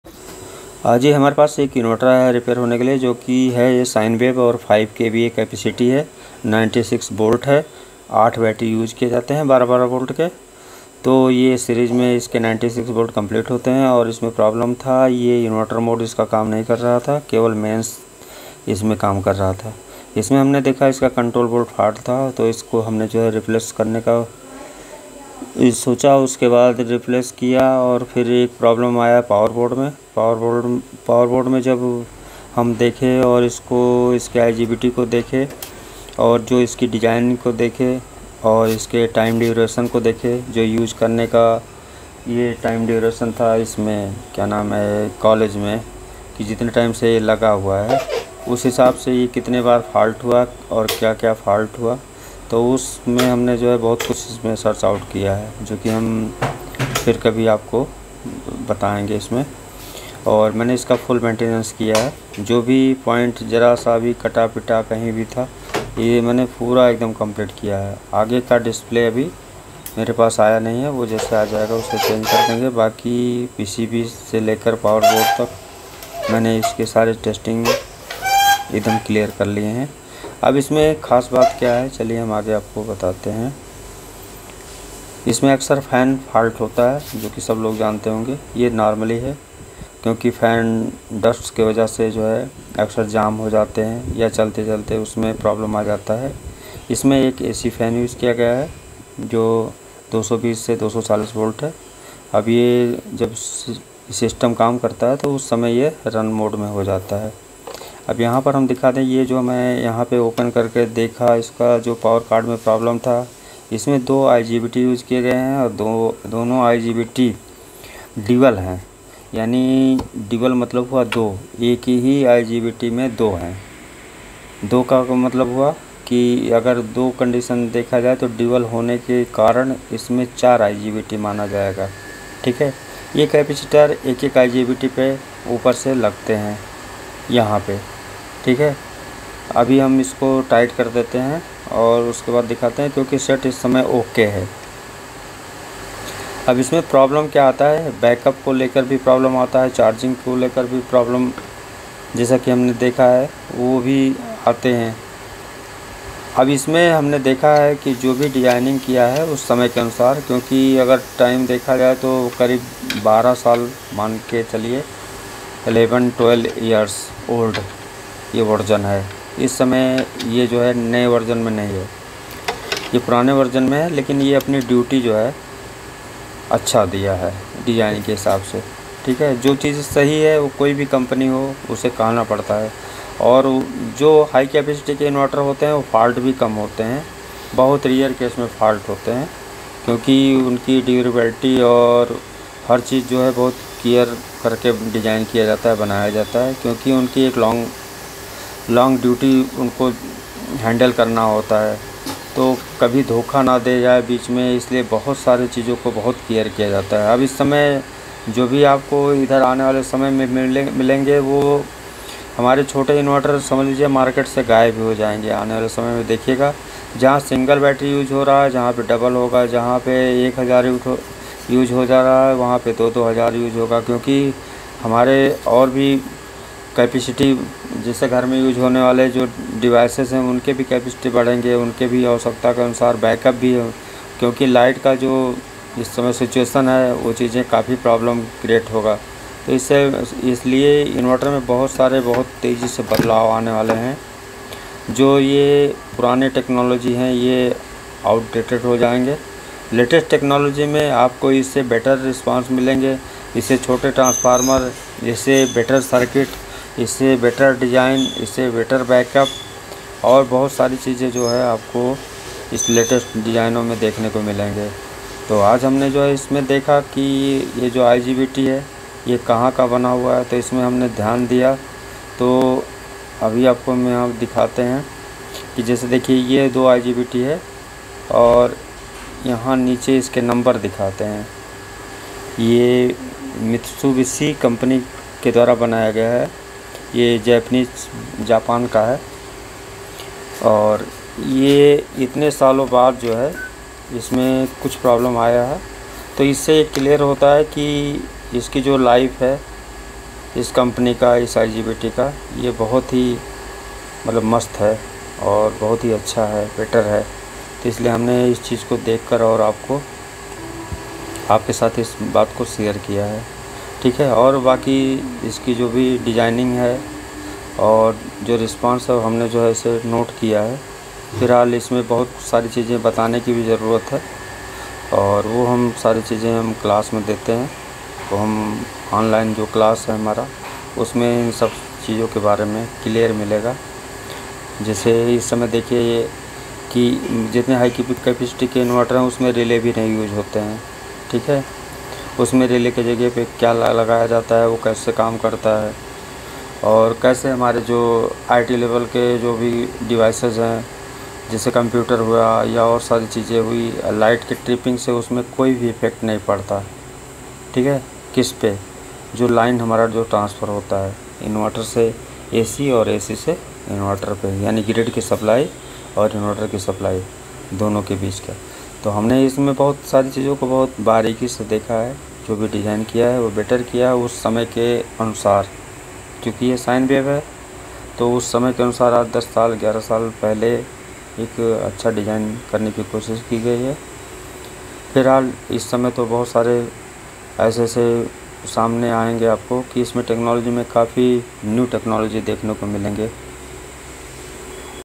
5KVA /96V luminous CRUZE sinewave inverter use for college office Repairing dc mode not working 🔥
5KVA /96V luminous CRUZE sinewave inverter use for college office Repairing dc mode not working